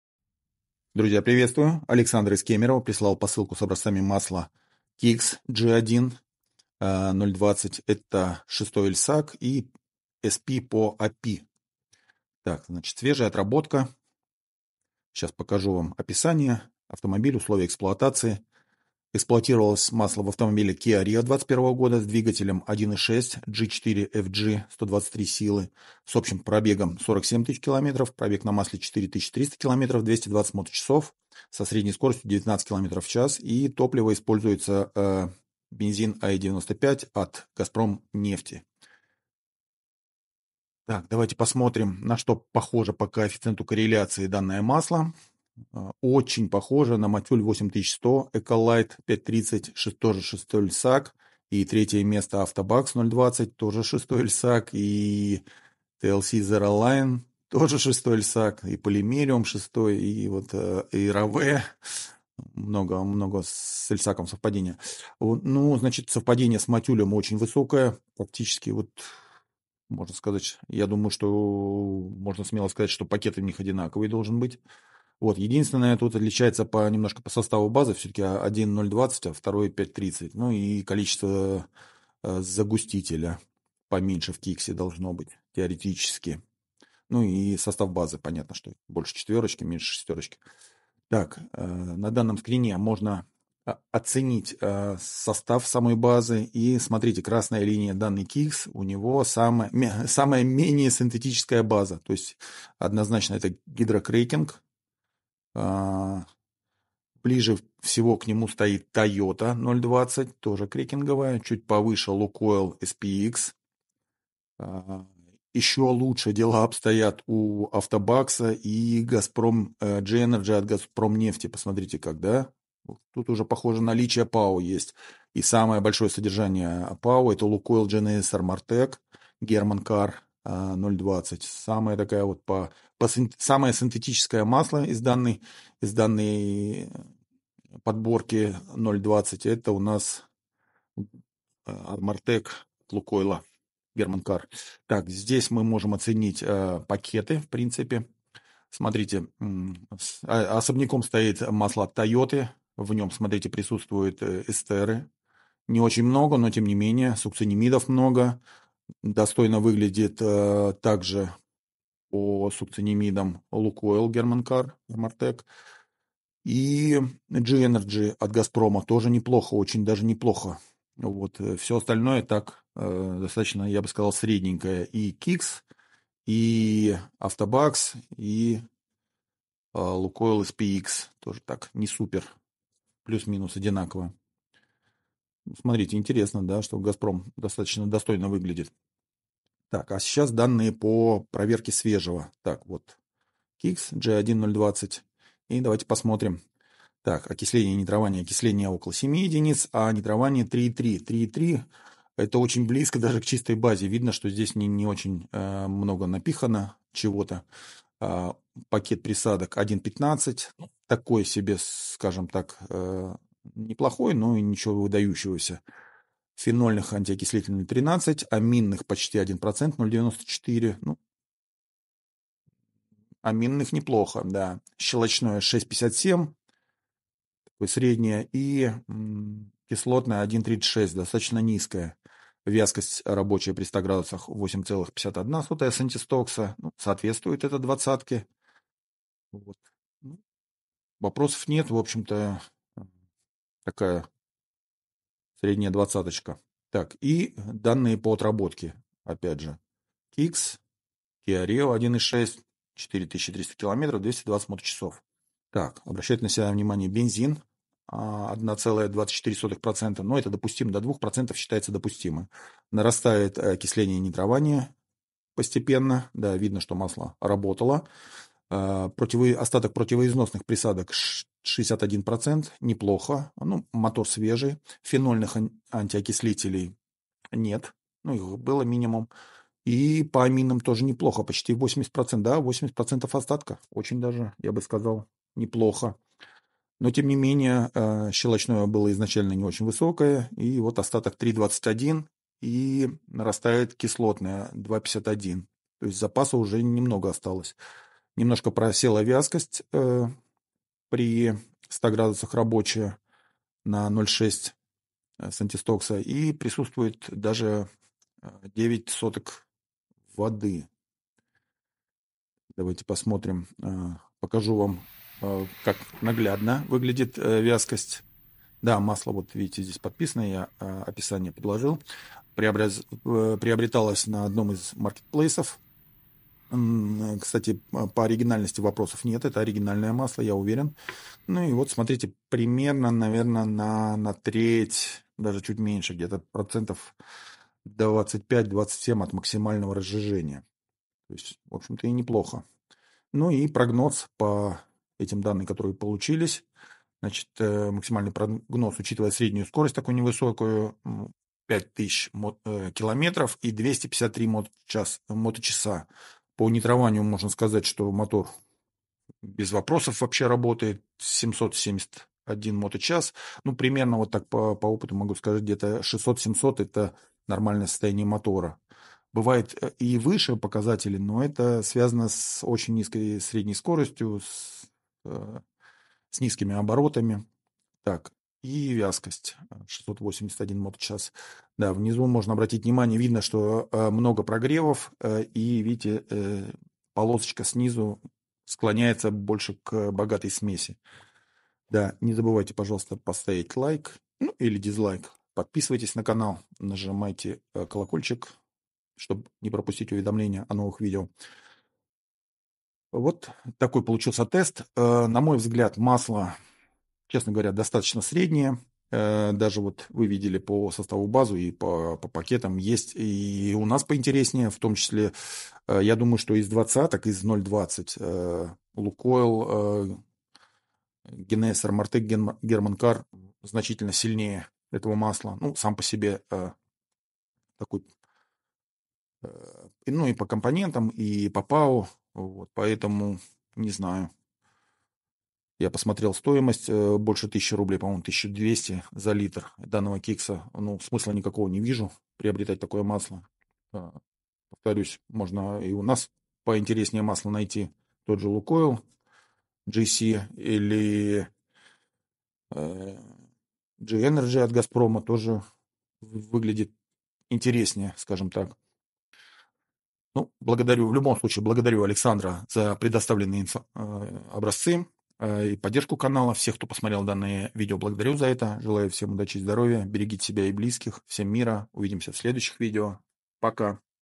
Kixx g1 0w20 (отработка из Kia Rio (G4FG) - 4 300 км., 220 моточасов)
Александр из Кемерово, прислал образцы масла Kixx G1 0w20 SP - GF-6.
Эксплуатировалось в Kia Rio, 2021 г.в.
с двигателем 1,6 (G4FG) - 123 силы.
пробег на масле 4 300 км., 220 моточасов
средняя скорость - 19 км/ч.
без доливок
городская эксплуатация, осень - зима, с большим количеством прогревов.
топливо; Газпромнефть АИ-95

Благодарю Александра за образцы и поддержку канала.

необходимое описание к образцам (в свободной форме)